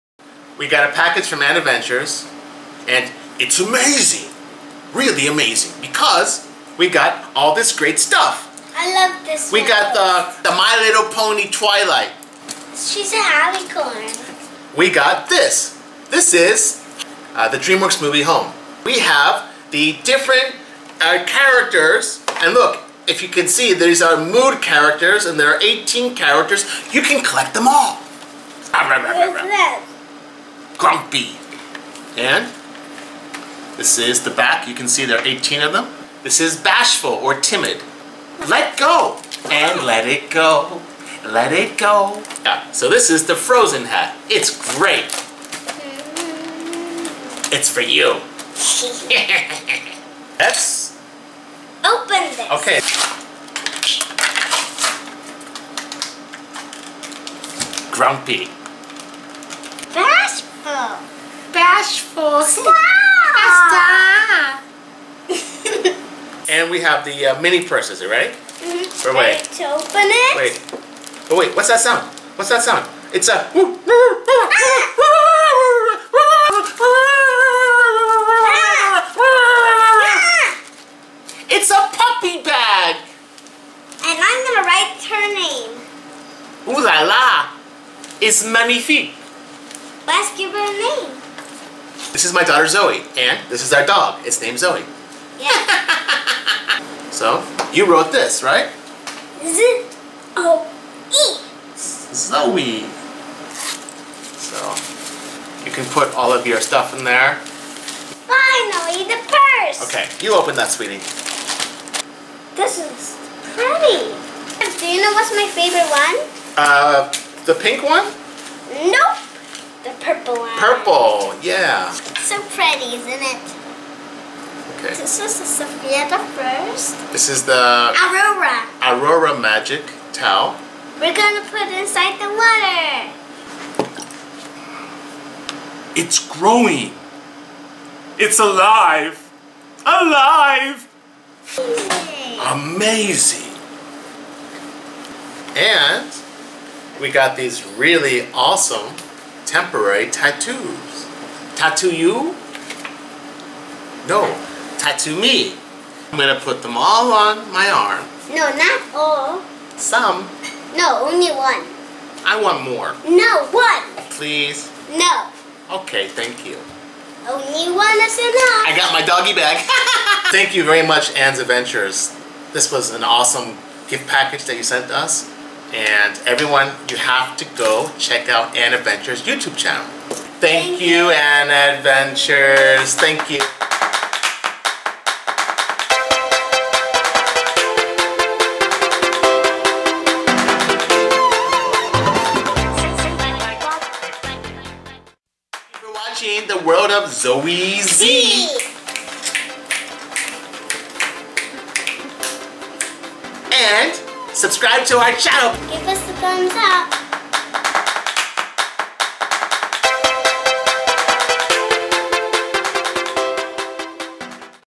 My Little Pony (MLP), Home, Littlest Pet Shop (LPS), etc
We got wonderful packages from Anneaventures, Evies Toy House, & ChristmasEveryDay.

Thanks guys! You all made our day, no... our week!!
Please enjoy our video and check out what we got from our wonderful Youtube friends!